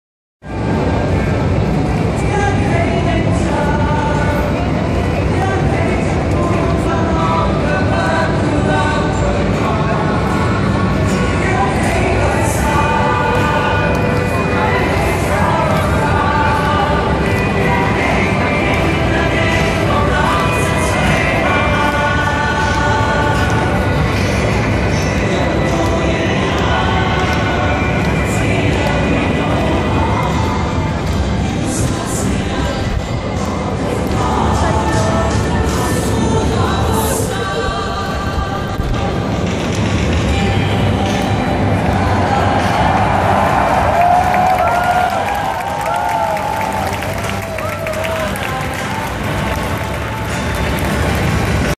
Peace rally in Admiralty 041014
Thousands of pro democracy activists sing along to songs at a rally after a day and night of violent antagonism from pro Beijing forces